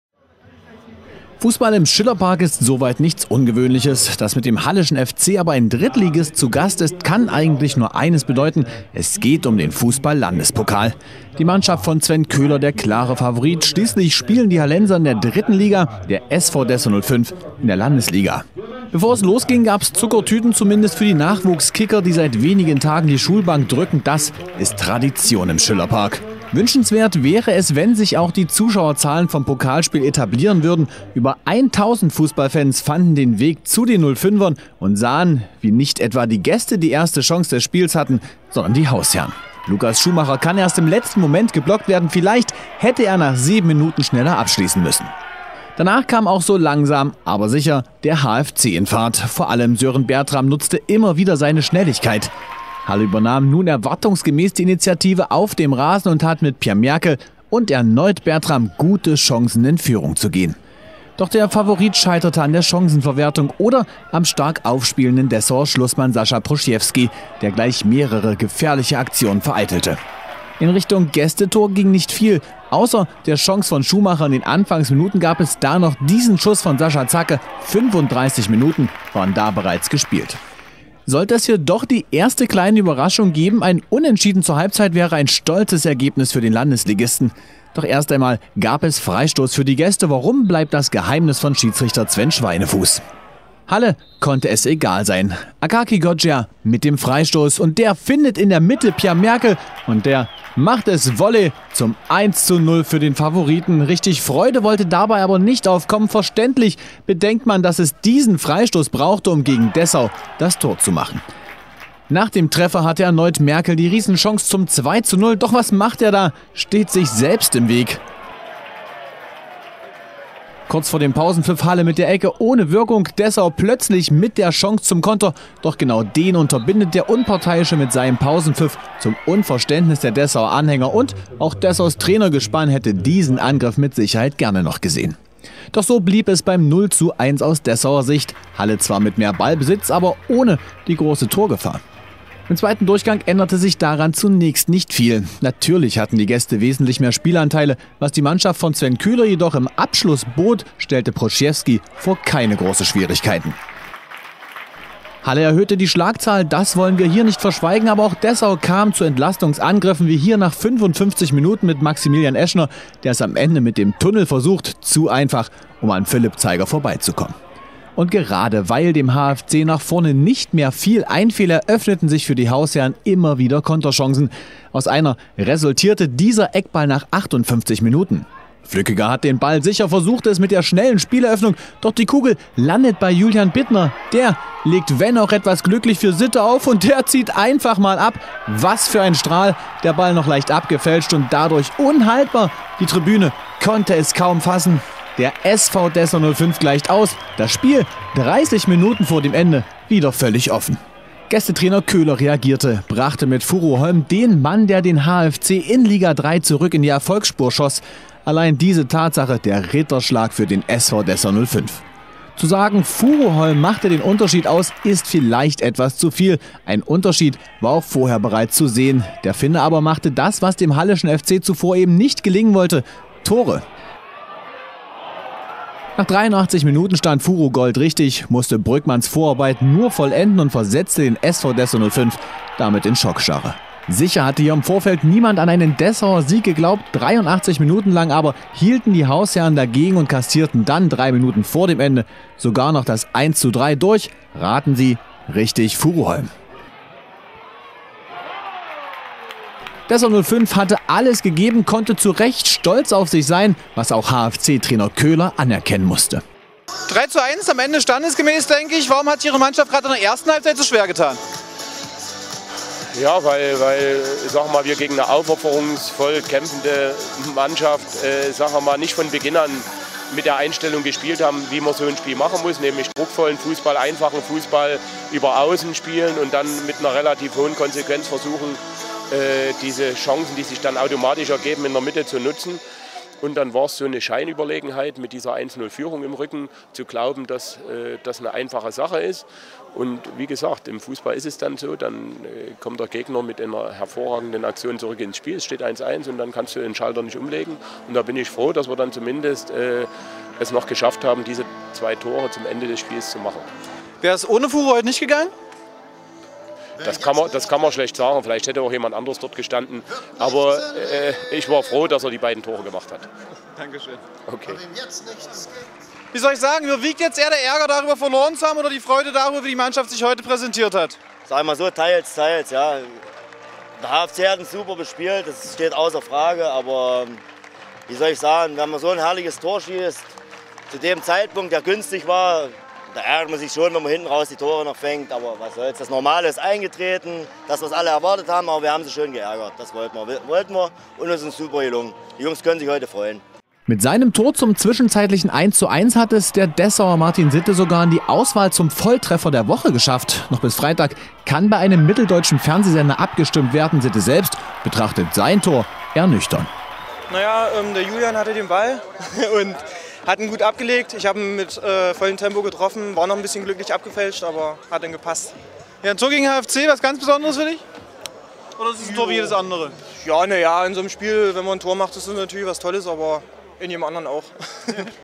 SV Dessau 05 gegen Halleschen FC - RAN1
Die erste Hauptrunde des Landespokal im Fußball hat dem SV Dessau 05 ein Traumlos beschert. Mit dem Halleschen FC kam der Pokalfavorit und Drittligist ins Stadion am Schillerpark. Am Ende setzte sich der Favorit durch, auch wenn er sich dabei nicht gerade mit Ruhm bekleckerte.